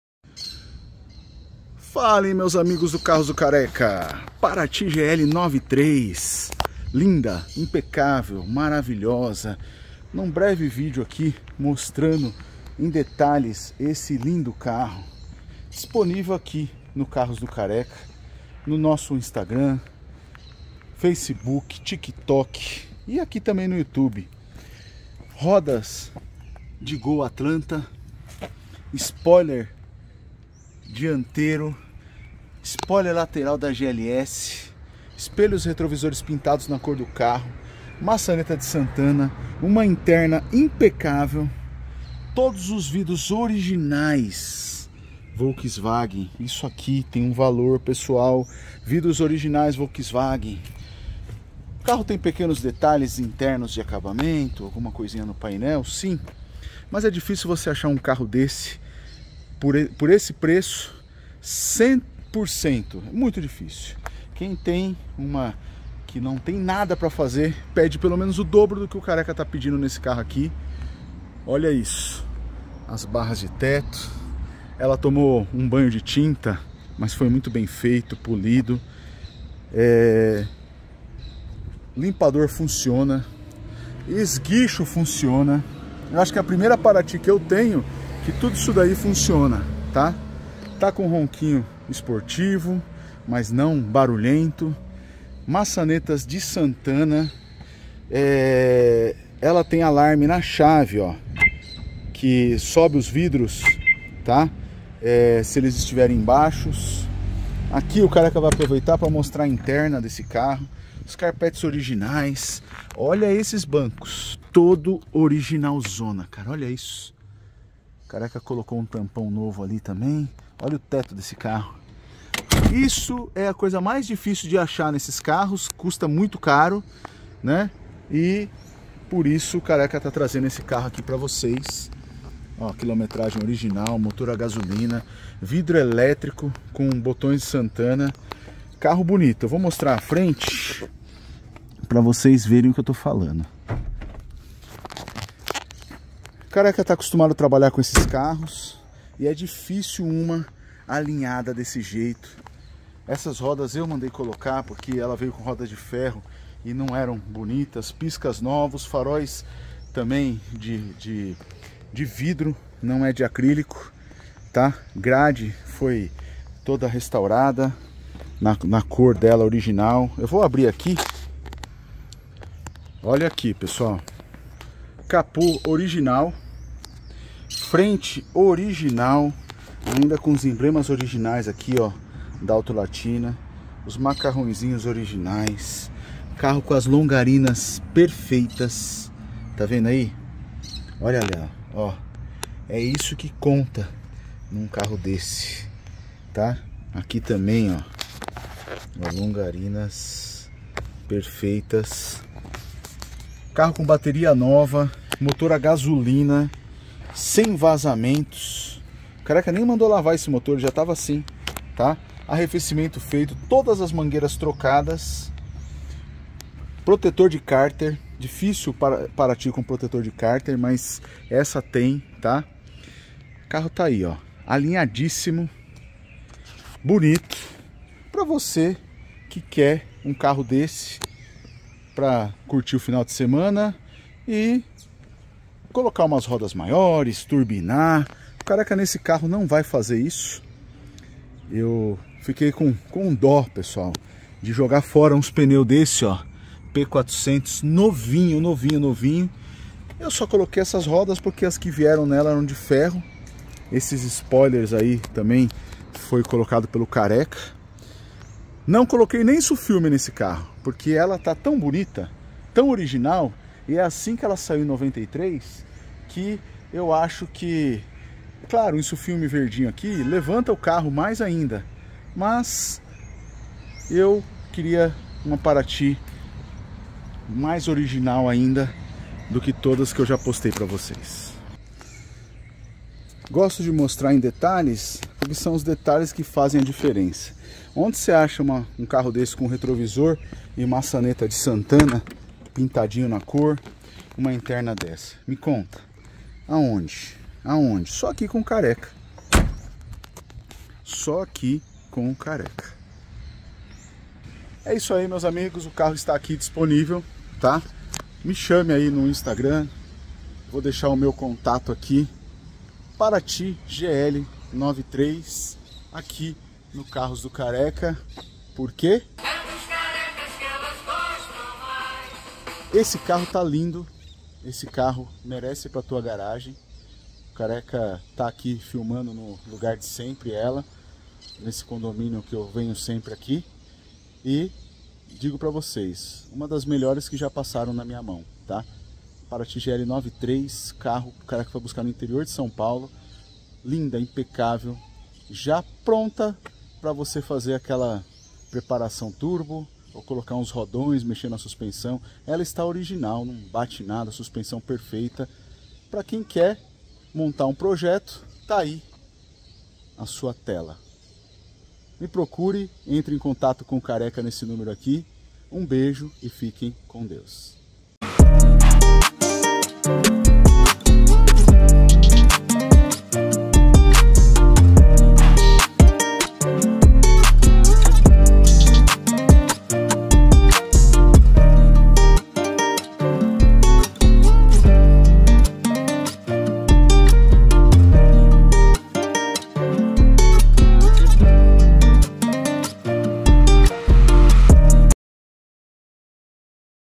Parati GL 1993 um clássico que marcou gerações e ainda hoje arranca suspiros dos entusiastas,A Venda
Quem foi jovem nos anos 90 desejava ir a praia com um carro desses que era conhecido como carro de surfista . Imagine levar as pranchas no rack e chegar a praia num desses era algo que todos queriam. Afora vc pode realizar seus sonhos e desejos, pois decidi colocar a venda minha Parati 1993 GL a gasolina. Chama o Careca no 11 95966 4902